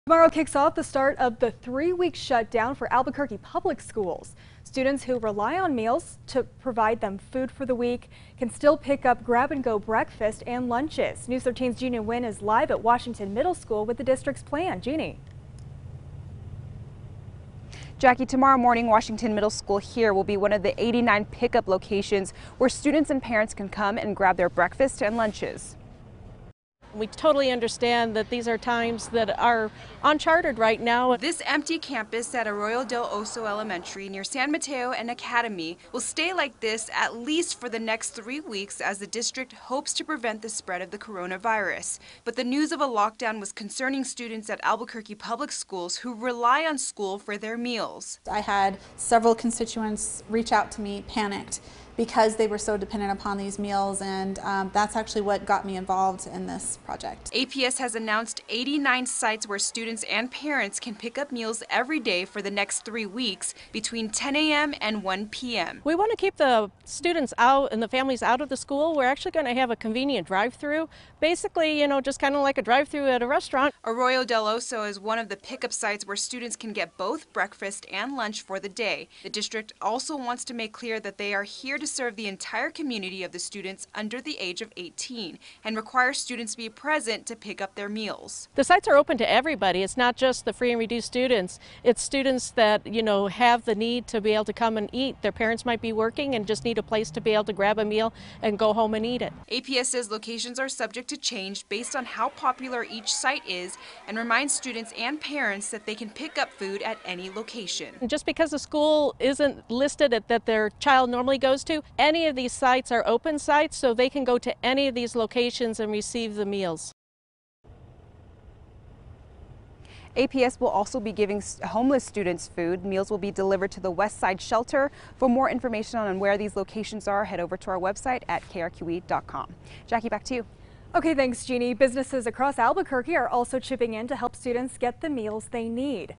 APS student meal pick-ups to start this week
Tomorrow kicks off the start of the three week shut down for Albuquerque Public SChools. Students who rely on school to provide meals can still pick up grab-and-go breakfasts and lunches.